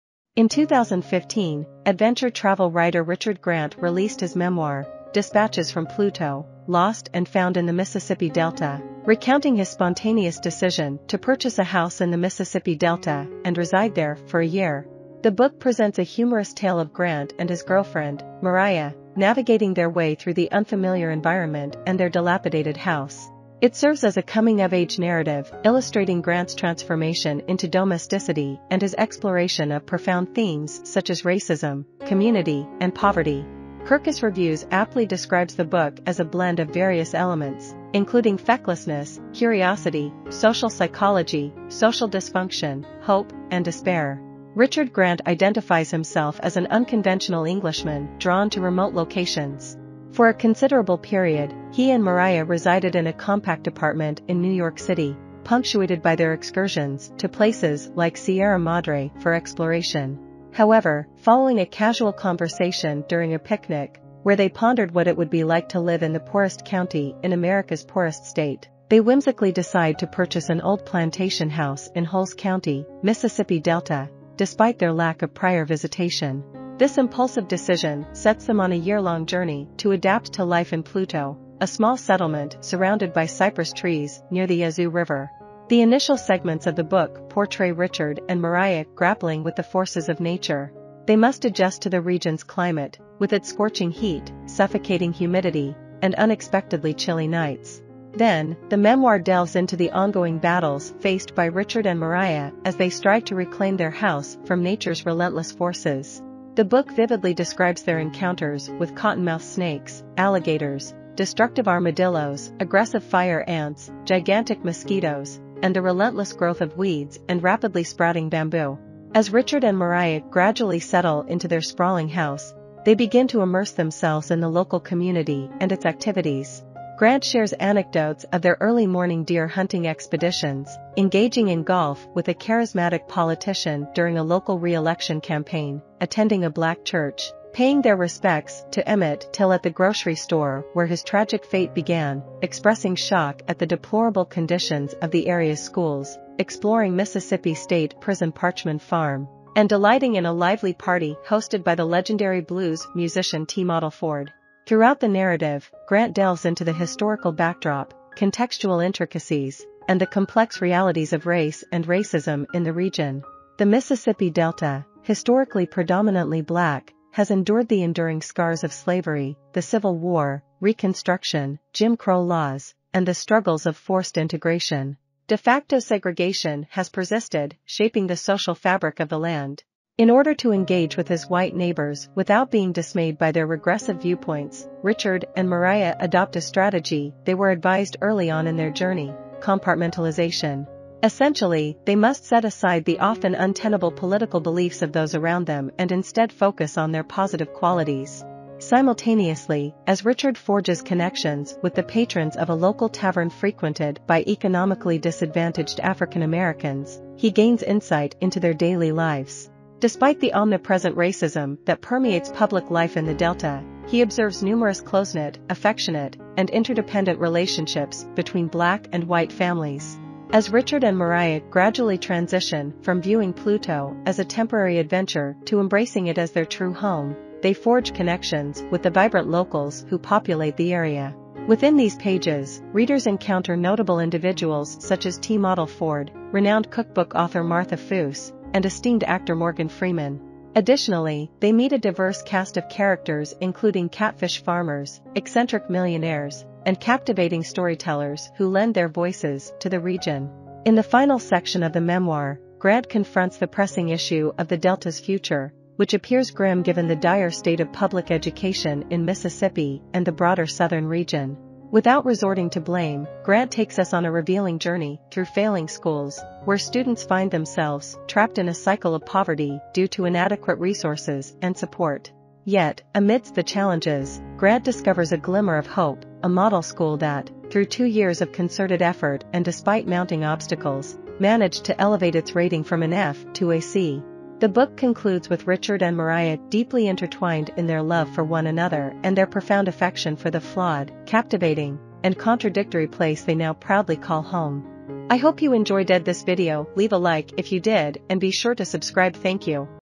Plot summary, “Dispatches from Pluto” by Richard Grant in 5 Minutes - Book Review
"Dispatches from Pluto: Lost and Found in the Mississippi Delta" is a non-fiction book written by Richard Grant. The book was published in 2015 and recounts Grant's experiences and observations as he and his girlfriend move from New York City to the rural Mississippi Delta.
In "Dispatches from Pluto," Richard Grant shares his journey of relocating to an area known for its poverty, racial tensions, and cultural complexities. Grant, a British journalist and author, provides a unique outsider's perspective as he immerses himself in the daily life of the Delta.
The book explores various aspects of the Mississippi Delta, including its history, social dynamics, and racial divisions. Grant encounters a wide range of characters, from wealthy landowners to impoverished locals, and from African American civil rights activists to white supremacists. Through his interactions and experiences, he delves into the complexities of race, class, and culture in this region.
Grant's writing style is often humorous and candid, allowing readers to gain insight into the challenges and triumphs of living in a place that is vastly different from his previous urban lifestyle. He explores the idiosyncrasies of the Delta, such as the region's obsession with hunting, the pervasive influence of religion, and the enduring legacies of the Civil War and the civil rights movement.
"Dispatches from Pluto" has been praised for its engaging storytelling, vivid descriptions, and Grant's ability to provide an authentic portrayal of the people and places he encounters. The book offers a nuanced perspective on the complexities of the Mississippi Delta, shedding light on the region's rich history, struggles, and resilience.
Overall, "Dispatches from Pluto" is an immersive and thought-provoking book that combines memoir, travelogue, and social commentary to offer a unique exploration of the Mississippi Delta and its cultural tapestry.